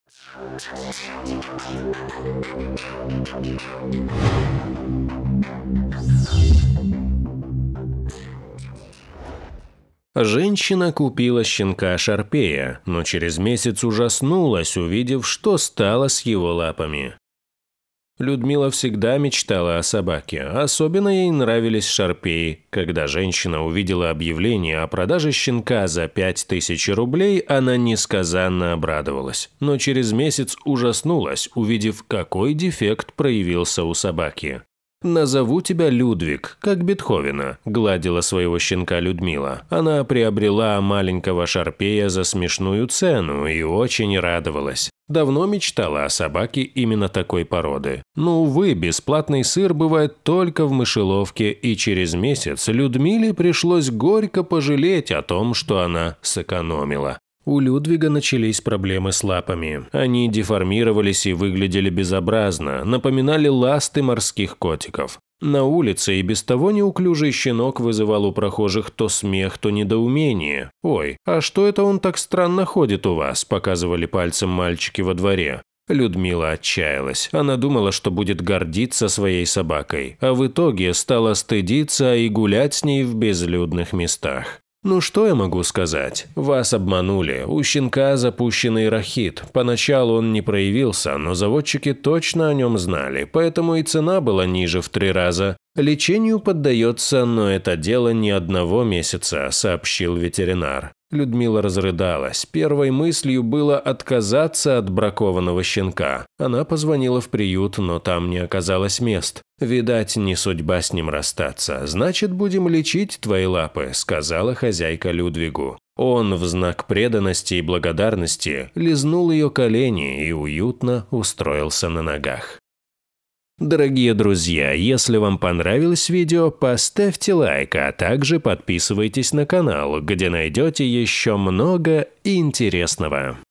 Женщина купила щенка шарпея, но через месяц ужаснулась, увидев, что стало с его лапами
Людмила всегда мечтала о собаке. Особенно ей нравились шарпеи. Когда женщина увидела объявление о продаже щенка за 5 тысяч рублей, она несказанно обрадовалась. Но через месяц ужаснулась, увидев, какой дефект проявился у собаки.